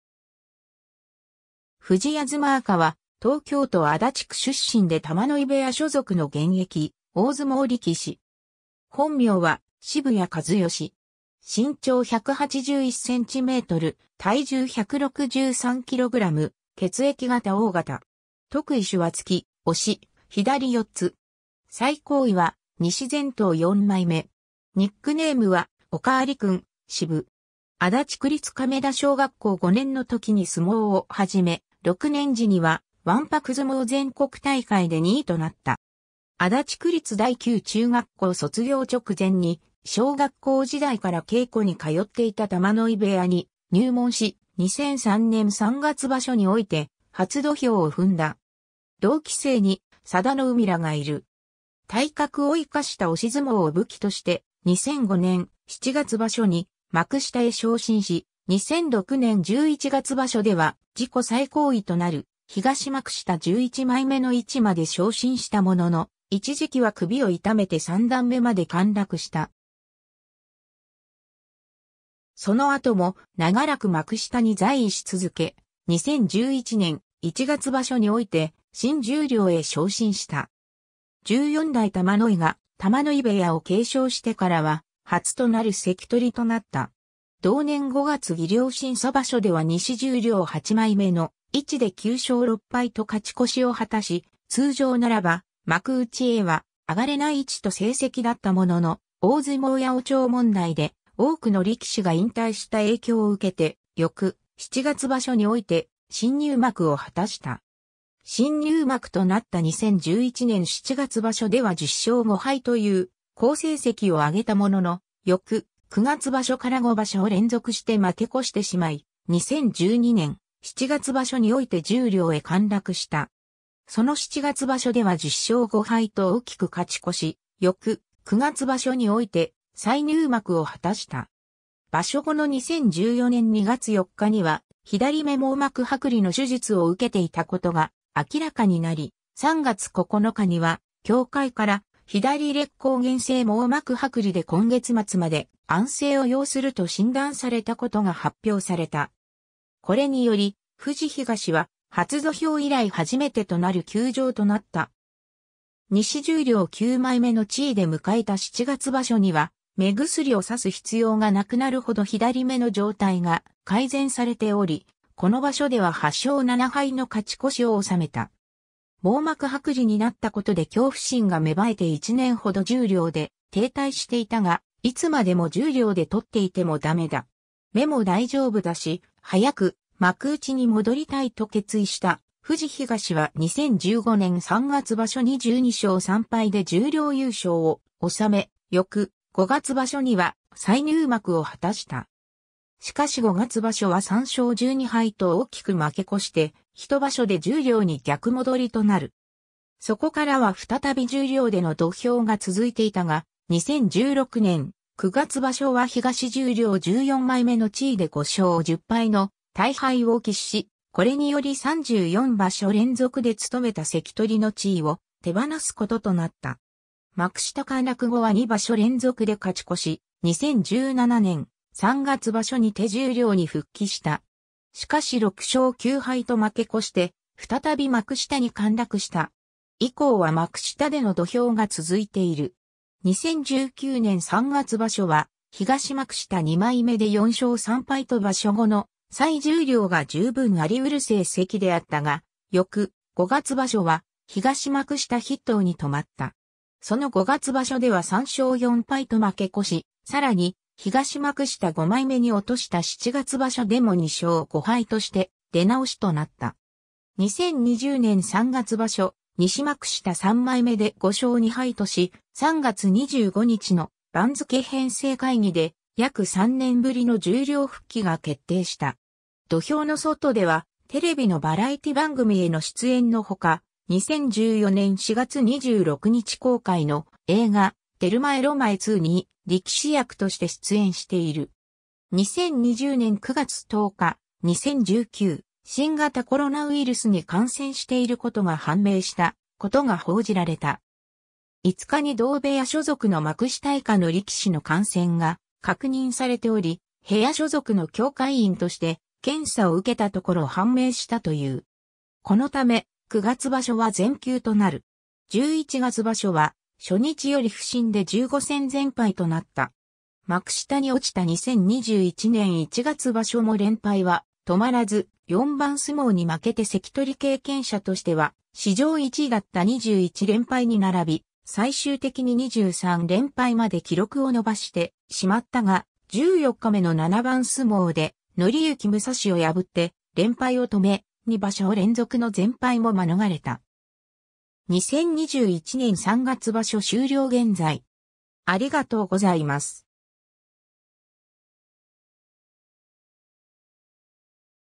富士東和佳
富士東 和佳（ふじあずま かずよし、1987年4月19日 - ）は、東京都足立区出身で玉ノ井部屋所属の現役大相撲力士。
本名は渋谷 和由（しぶや かずよし）。
身長181cm、体重163kg、血液型O型。
得意手は突き、押し、左四つ。
最高位は西前頭4枚目（2013年7月場所）。
ニックネームは「（角界の）おかわり君」、シブ。
 足立区立亀田小学校5年の時に相撲を始め、6年時にはわんぱく相撲全国大会で2位となった。
足立区立第九中学校卒業直前に、小学校時代から稽古に通っていた玉ノ井部屋に入門し、2003年3月場所において初土俵を踏んだ。
同期生に佐田の海らがいる。
 体格を生かした押し相撲を武器として、2005年7月場所に幕下へ昇進し、2006年11月場所では自己最高位となる東幕下11枚目の位置まで昇進したものの、一時期は首を痛めて三段目まで陥落した。
その後も長らく幕下に在位し続け、2011年1月場所において新十両へ昇進した。
14代玉ノ井（元大関・栃東）が玉ノ井部屋を継承してからは初となる関取となった。
同年5月技量審査場所では西十両8枚目の位置で9勝6敗と勝ち越しを果たし、通常ならば幕内へは上がれない位置と成績だったものの、大相撲八百長問題で多くの力士が引退した影響を受けて、翌7月場所において新入幕を果たした。
 新入幕となった2011年7月場所では10勝5敗という好成績を挙げたものの、翌9月場所から5場所連続して負け越してしまい、2012年7月場所において十両へ陥落した。
その7月場所では10勝5敗と大きく勝ち越し、翌9月場所において再入幕を果たした。
場所後の2014年2月4日には左目網膜剥離の手術を受けていたことが明らかになり、3月9日には協会から「左裂孔原性網膜剥離」で今月末まで安静を要すると診断されたことが発表された。
これにより富士東は初土俵以来初めてとなる休場となった。
西十両9枚目の地位で迎えた7月場所には目薬を差す必要が無くなるほど左目の状態が改善されており、この場所では8勝7敗の勝ち越しを治めた。
網膜剥離になったことで恐怖心が芽生えて1年ほど十両で停滞していたが「いつまでも十両で取っていてもだめだ。目も大丈夫だし、早く幕内に戻りたい」と決意した富士東は2015年3月場所に12勝3敗で十両優勝を収め、翌5月場所には再入幕を果たした。
しかし5月場所は3勝12敗と大きく負け越して一場所で十両に逆戻りとなる。
そこからは再び十両での土俵が続いていたが、2016年9月場所は東十両14枚目の地位で5勝10敗の大敗を喫し、これにより34場所連続で務めた関取の地位を手放すこととなった。
幕下陥落後は2場所連続で勝ち越し、2017年3月場所にて十両に復帰した。
しかし6勝9敗と負け越して再び幕下に陥落した。
以降は幕下での土俵が続いている。
2019年3月場所は東幕下2枚目で4勝3敗と場所後の再十両が十分有り得る成績であったが、翌5月場所は東幕下筆頭に留まった。
その5月場所では3勝4敗と負け越し、さらに東幕下5枚目に落とした7月場所でも2勝5敗として出直しとなった。
2020年3月場所、西幕下3枚目で5勝2敗とし、3月25日の番付編成会議で約3年ぶりの十両復帰が決定した。
 土俵の外では、テレビのバラエティ番組への出演の他、2014年4月26日公開の映画『テルマエ・ロマエII』に、力士役として出演している。
 2020年9月10日、2019新型コロナウイルスに感染していることが判明したことが報じられた。
5日に同部屋所属の幕下以下の力士の感染が確認されており、部屋所属の協会員として検査をうけたところ判明したという。
このため、9月場所は全休となる。
11月場所は初日より不振で15戦全敗となった（前の9月場所では王輝が十両で15戦全敗を喫しており、十両で2場所連続で15戦全敗力士が出るのは史上初めてとなる）。
 幕下に落ちた2021年1月場所も連敗は止まらず、4番相撲に負けて関取経験者としては史上1位だった21連敗に並び、最終的に23連敗まで記録を伸ばしてしまった（2020年7月場所14日目から）が、14日目の7番相撲で徳之武藏を破って連敗を止め、2場所連続の全敗も免れた。
 2021年3月場所終了現在